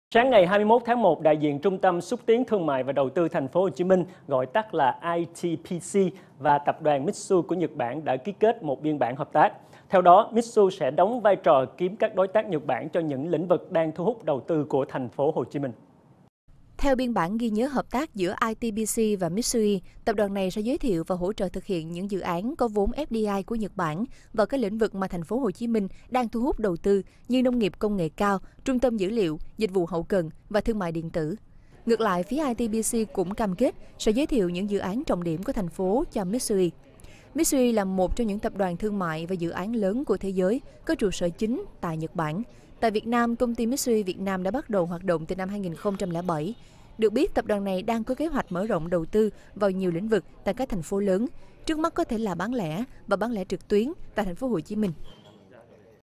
TP.HCM ký biên bản hợp tác với Mitsui
Sáng ngày 21/1, đại diện Trung tâm Xúc tiến Thương mại và Đầu tư Thành phố Hồ Chí Minh, gọi tắt là ITPC và tập đoàn Mitsui của Nhật Bản đã ký kết một biên bản hợp tác. Theo đó, Mitsui sẽ đóng vai trò tìm kiếm các đối tác Nhật Bản cho những lĩnh vực đang thu hút đầu tư của thành phố Hồ Chí Minh.